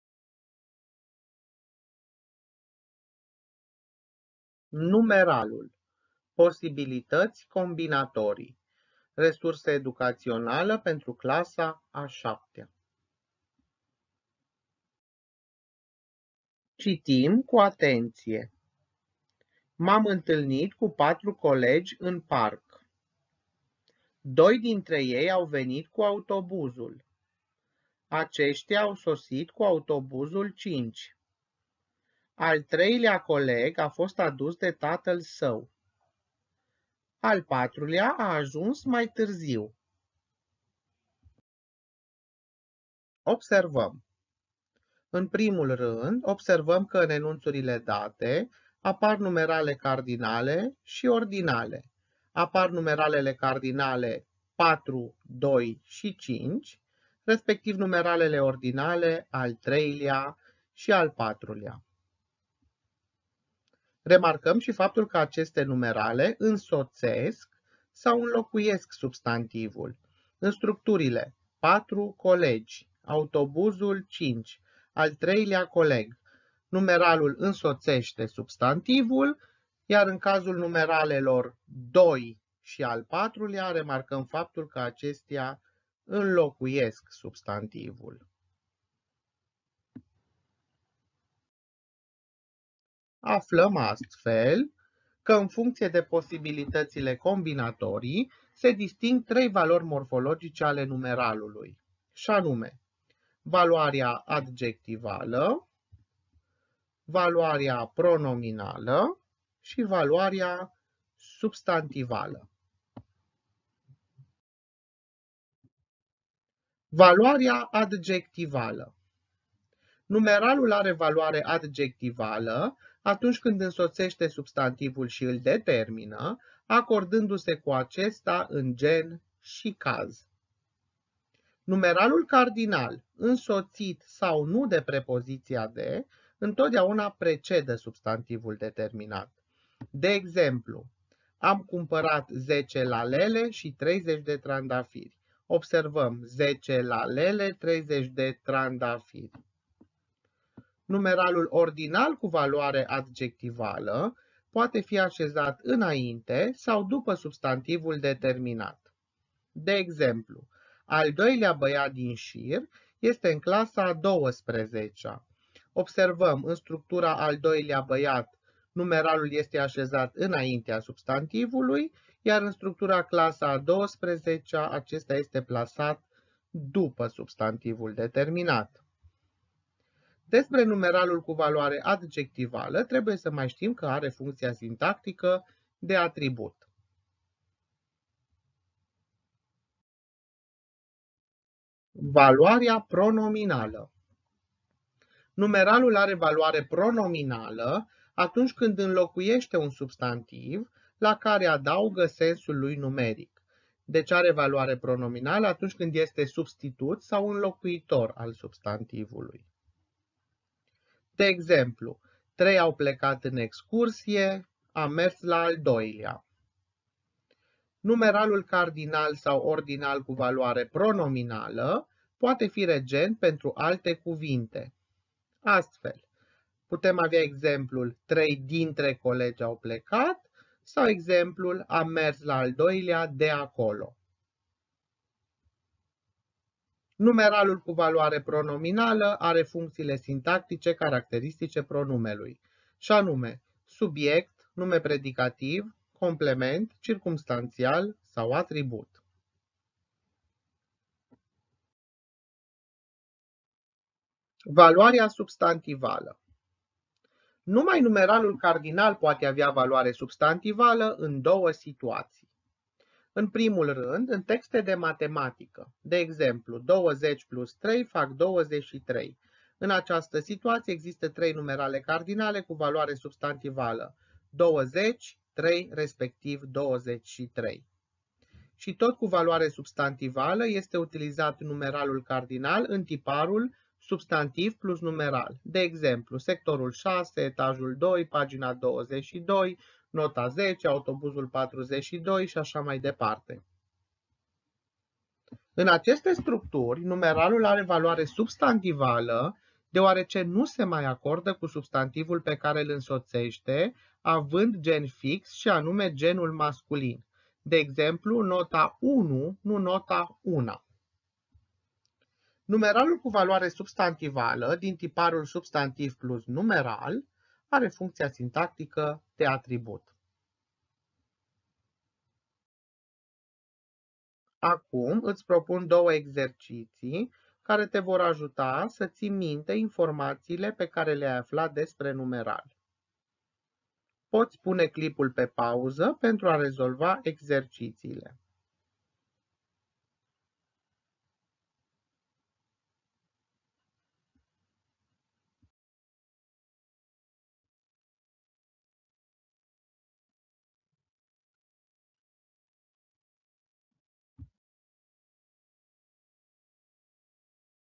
Numeralul - posibilități combinatorii
În această resursă sunt tratate posibilitățile combinatorii ale numeralului, respectiv valorile morfologice ale acestuia. În prima parte a resursei, se pornește de la exemple de enunțuri în care sunt utilizate numerale cardinale și ordinale în diverse combinații, cu cele 3 valori morfologice, acestea fiind evidențiate ulterior. Următoarea parte a resursei este dedicată prezentării celor trei valori ale numeralului: adjectivală, pronominală și substantivală, fiind oferite principalele observații referitoare la acestea, respectiv exemple corespunzătoare.
Informațiile științifice sunt preluate din lucrarea „Gramatica limbii române pentru gimnaziu”, coordonată de Gabriela Pană Dindelegan.După prezentarea aspectelor teoretice și după explicațiile susținute prin exemple, în cadrul resursei sunt incluse aplicații, cu scopul de a fi asigurate retenția și transferul.
Cuvinte-cheie: numeralul, posibilități combinatorii, valori morfologice, aplicații.

Autor:  PETRU BUCURENCIU